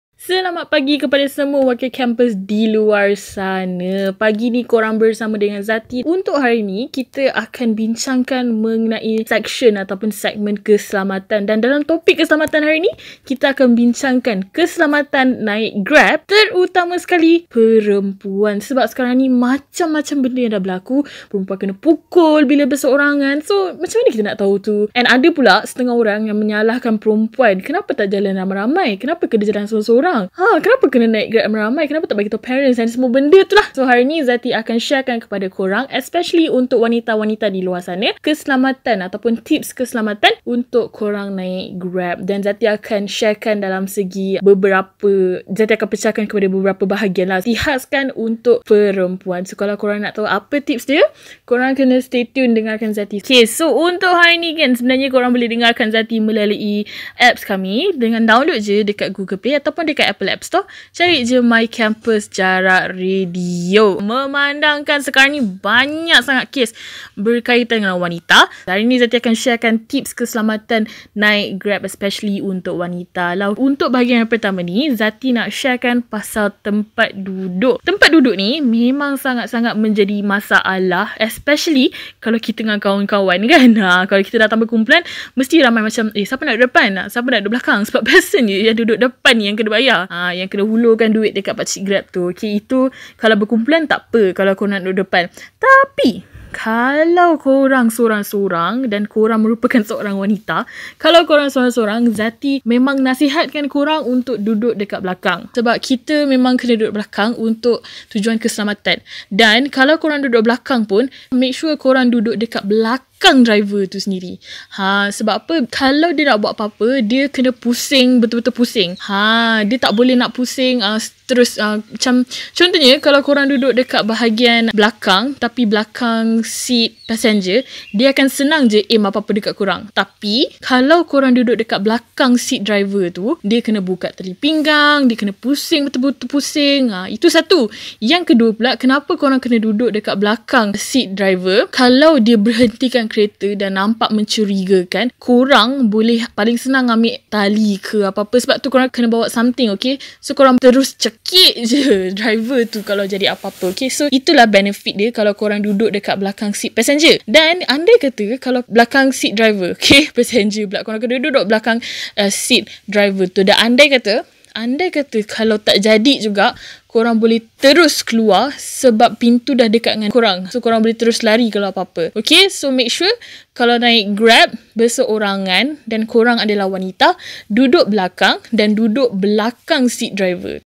Keselamatan Grab Wanita - Duduk Belakang Driver Untuk Selamat
Antara tips yang paling penting untuk korang especially wanita ialah korang mesti duduk di belakang driver. Ingat! Anda ialah customer. Duduk di belakang seat driver dan ia akan membataskan pergerakkan driver tersebut atau serangan daripada driver. Kalau driver nak serang, ambil seatbell yang dipakai dan lilit pada leher driver. Untuk maklumat lanjut, dengarkan podcast ini.